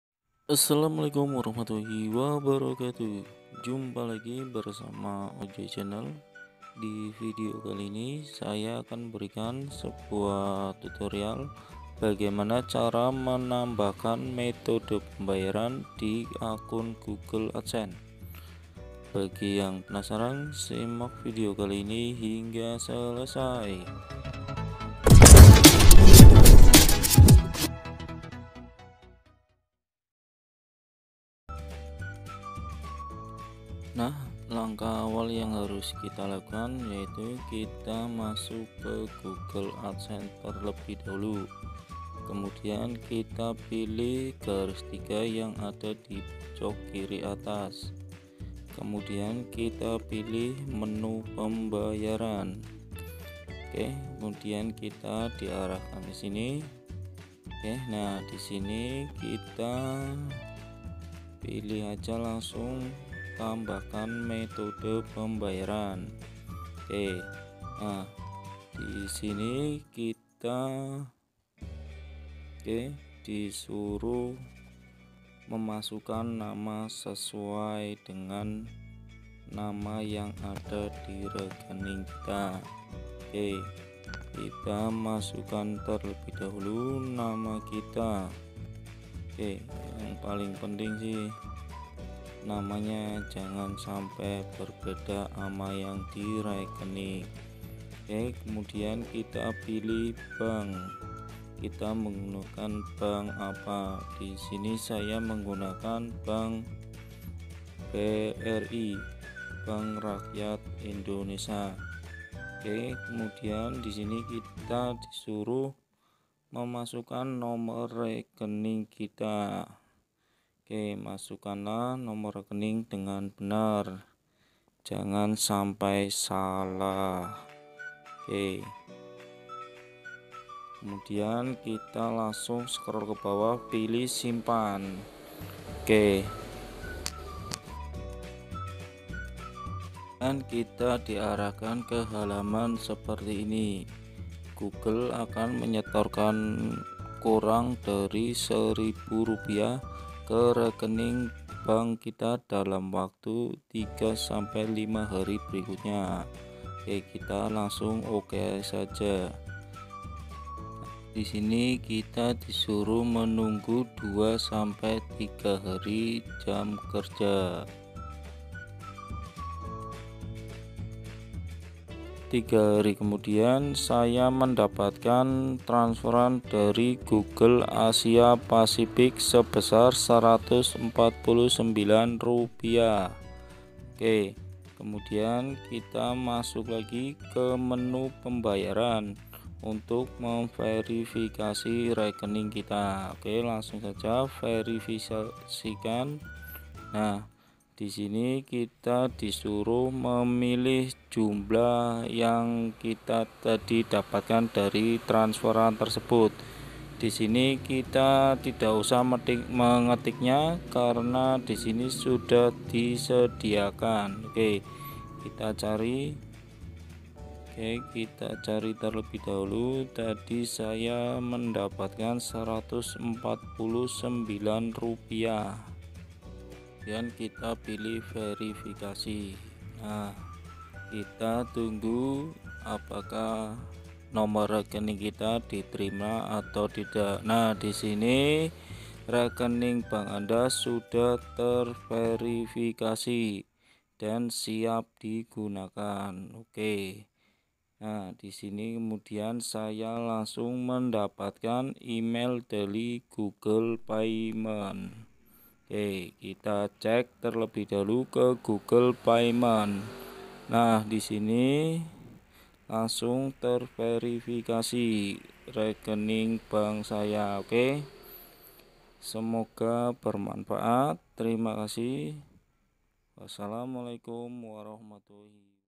cara menambahkan metode pembayaran di google adsense
di vidio kali ini saya akan memberikan cara menambahkan metode pembayaran google adsesnse dengan benar bagi yang penasaran simak vidio kali ini hingga selasai..